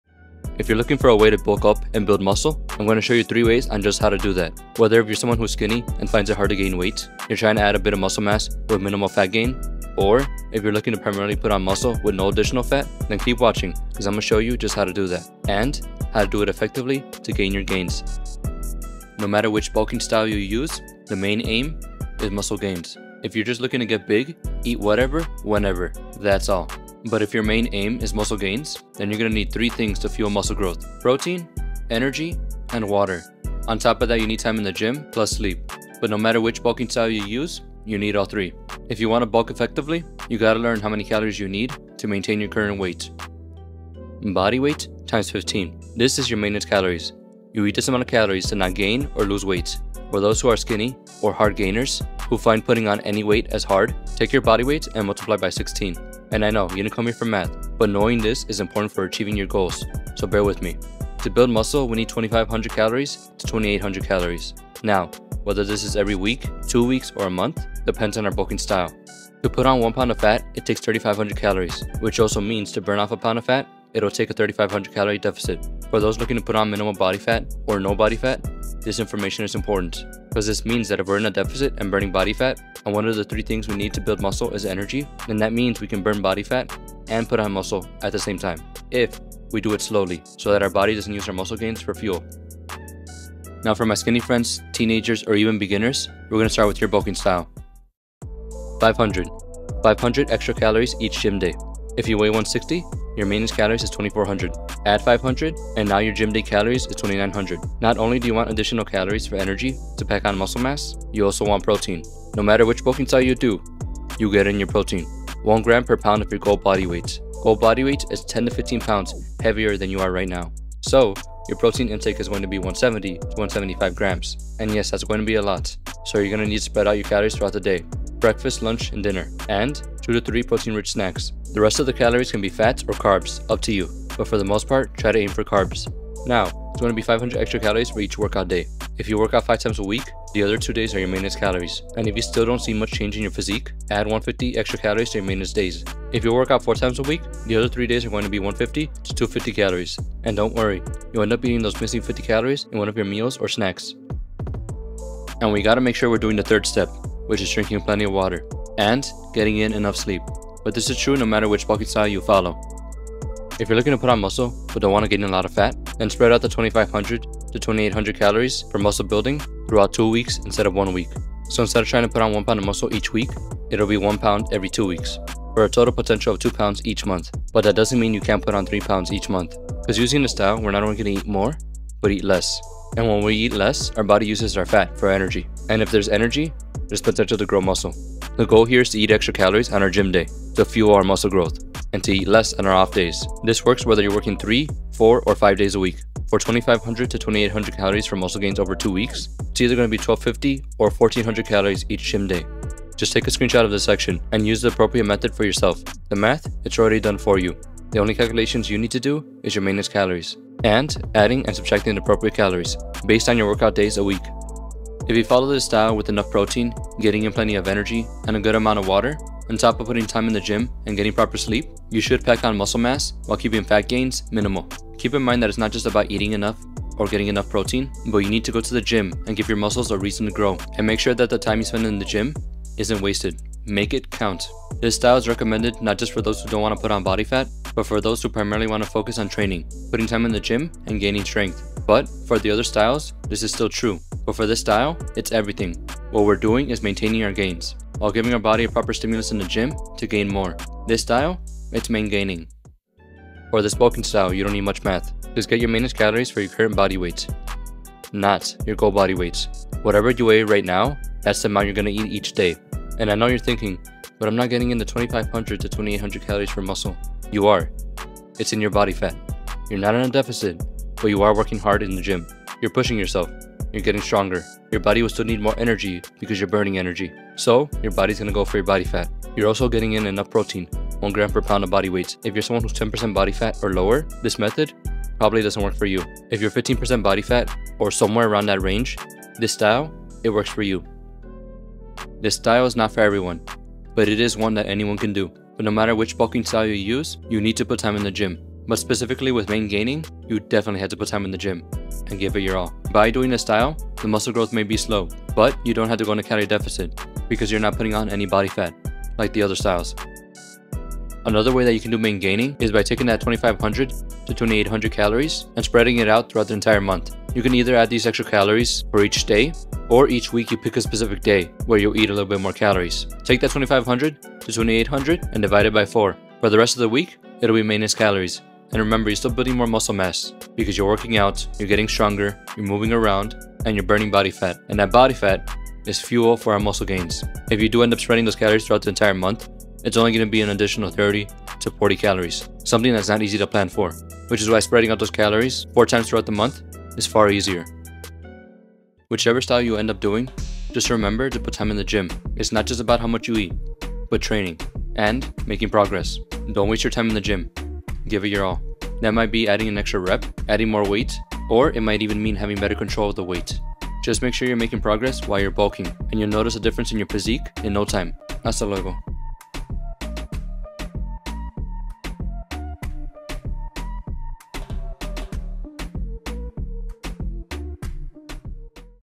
How to Successfully Bulk Up in 3 Months | Physical Fitness
If you’re someone struggling to put on any weight or you’re looking for a way to pack on muscle size
This video's gonna show 3 ways to go about doing just that

00:00 - Intro

00:44 -- Calories

01:38 - Mass Gaining

02:43 - Recomp Bulking

03:56 - MainGaining

05:11 - MainGaining pt 2

5:50 - End